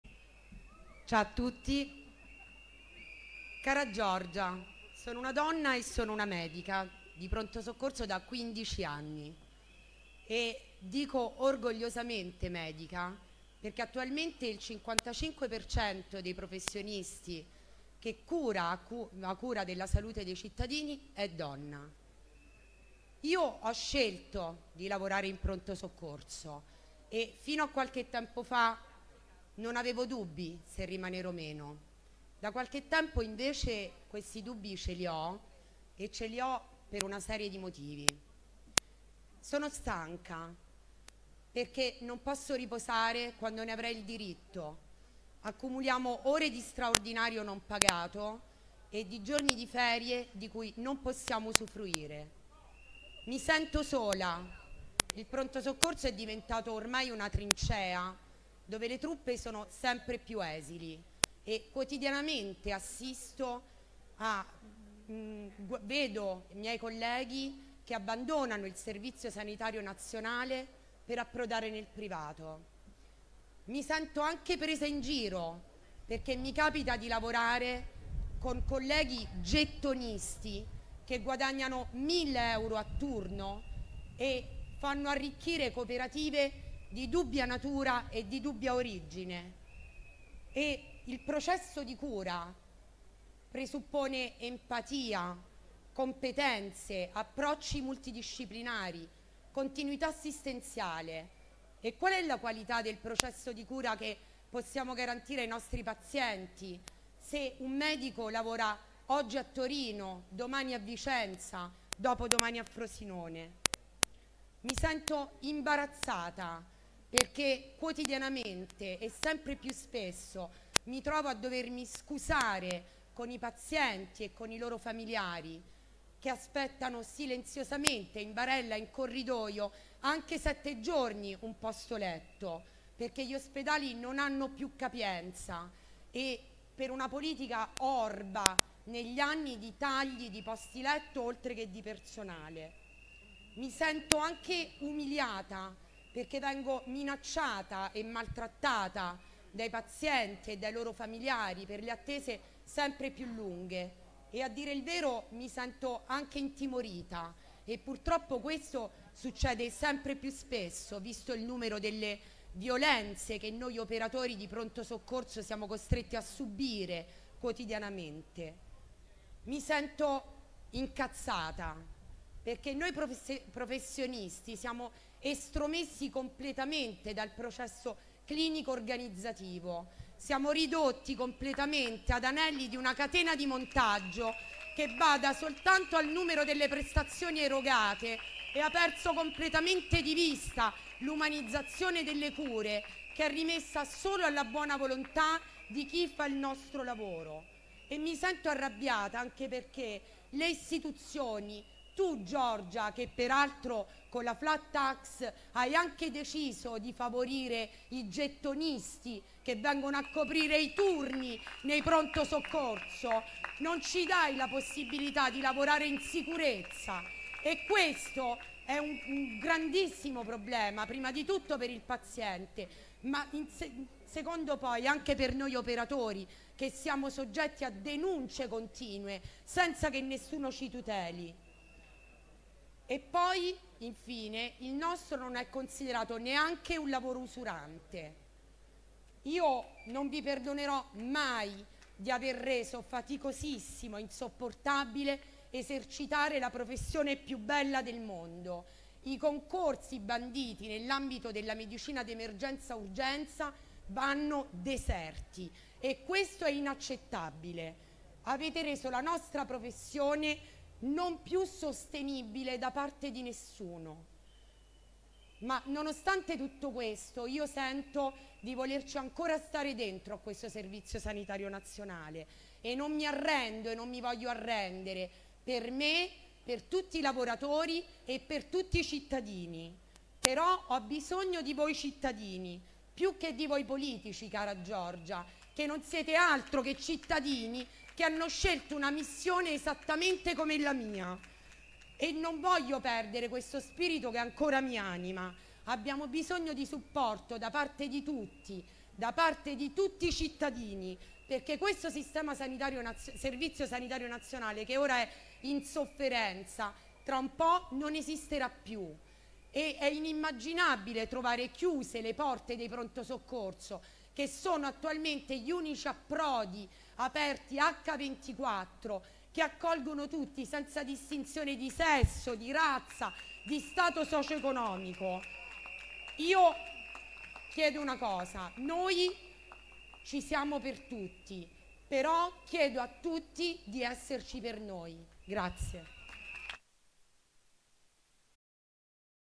Salviamo la Sanità Pubblica, le parole della medica Caterina
Caterina, medica in un Pronto Soccorso romano e delegata della Fp Cgil Medici e Dirigenti Ssn, legge una lettera indirizzata al presidente del Consiglio, Giorgia Meloni, dal palco della manifestazione di medici, veterinari e dirigenti sanitari, promossa dall’intersindacale ‘Uniti per la Sanità’, del 15 dicembre a Roma dietro le parole ‘Salviamo la Sanità Pubblica’.